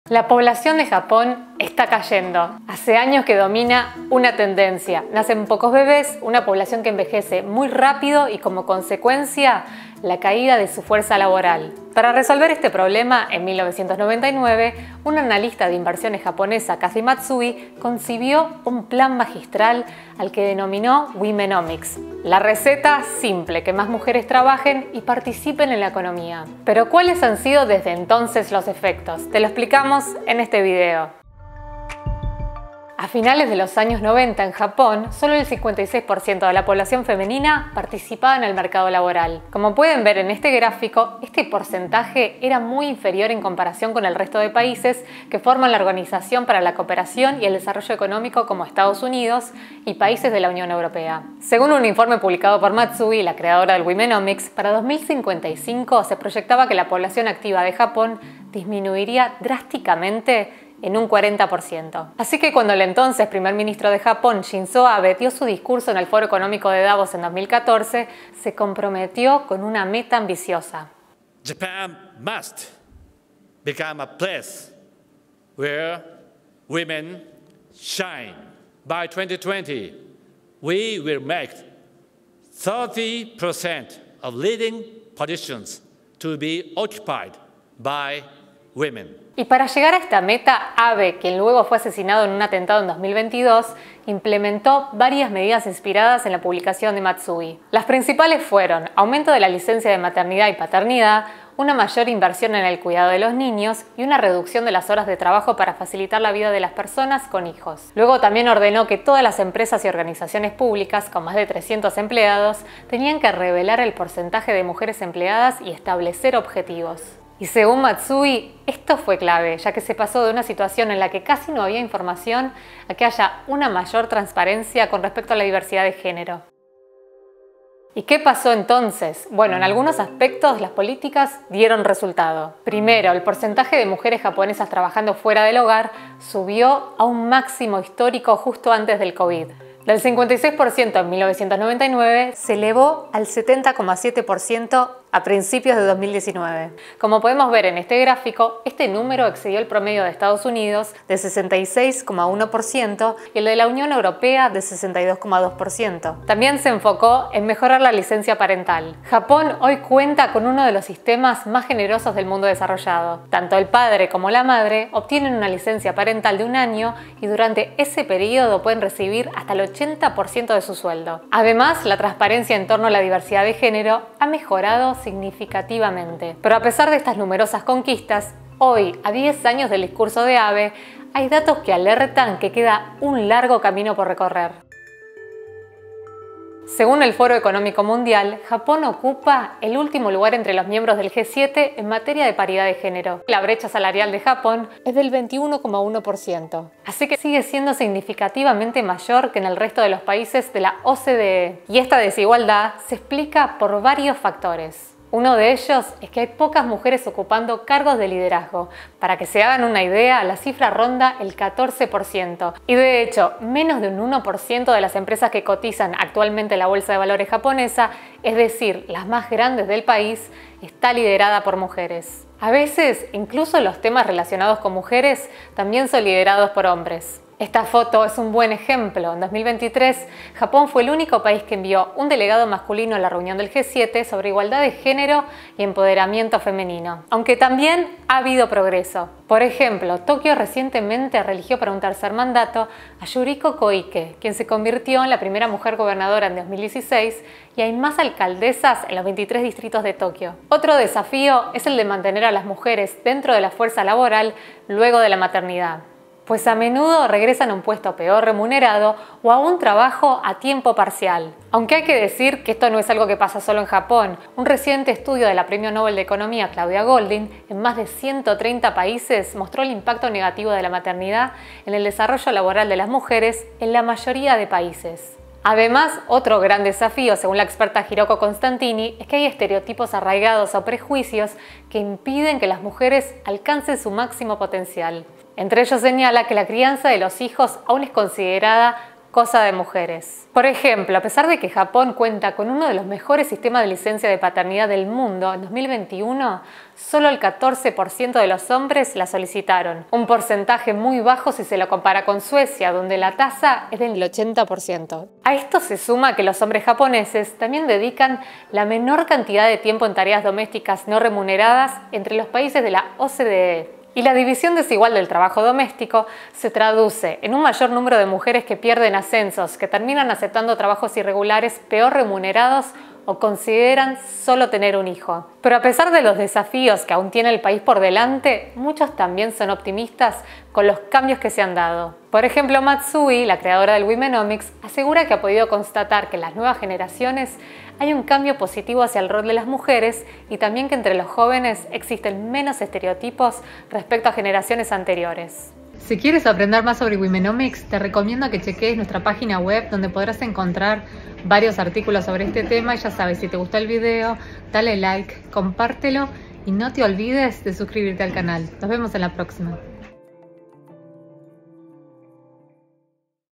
"Womenomics": qué efectos ha tenido en Japón el esfuerzo para que más mujeres se sumen a la economía
En 1999, la japonesa  Kathy Matsui concibió un plan magistral que prometía resolver uno de los desafíos más difíciles que enfrentaba la economía de su país: la reducción drástica de su fuerza laboral debido al nacimiento de pocos niños y el rápido envejecimiento de la población.

La visionaria idea de la analista de inversiones japonesa para ayudar a su país fue aprovechar el talento de la mitad de la población: las mujeres.

En este video te explicamos en qué consiste womenomics y cuáles han sido sus efectos.

● Presentación, guion y edición de video: Agustina Latourrette
● Investigación: Mariko Oi
● Editora: Ana María Roura